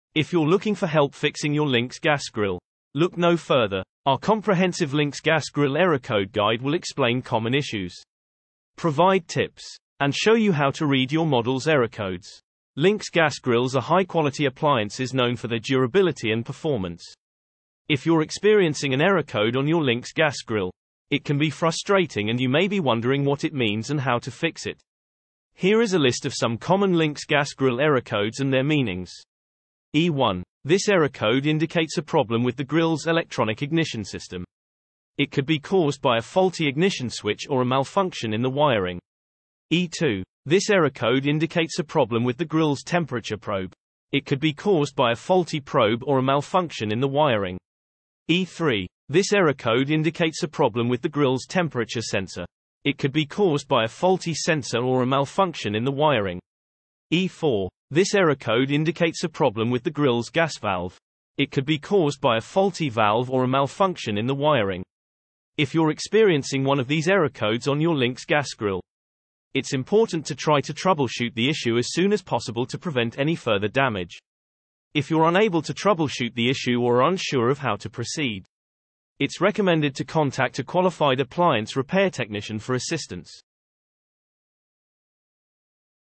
Lynx Gas Grill Error Codes
If you're looking for help fixing your Lynx Gas Grill, look no further! Our comprehensive Lynx Gas Grill error code guide will explain common issues, provide tips, and show you how to read your model's error codes.